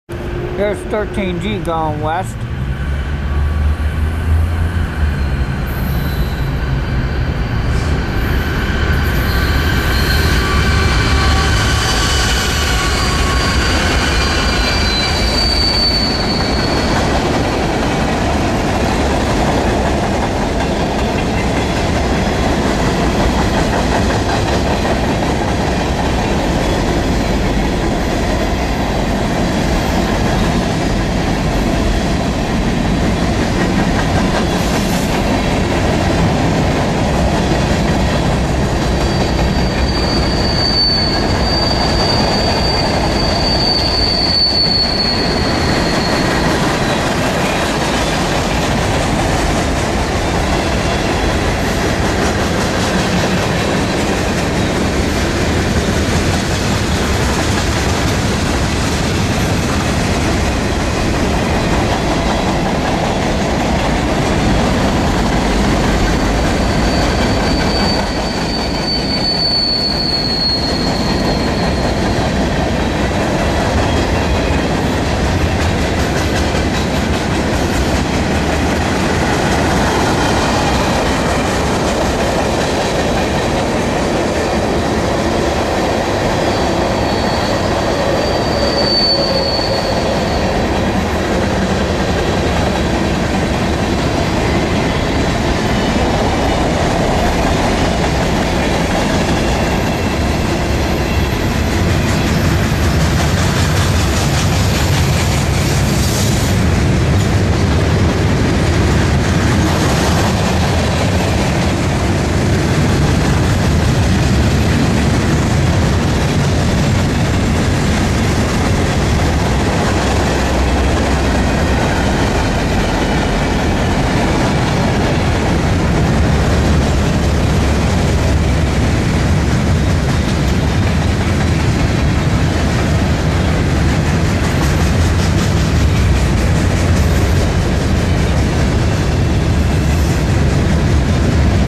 Brian's RR video's.
My Tank Train Video Collection.  This is the 13G heading west.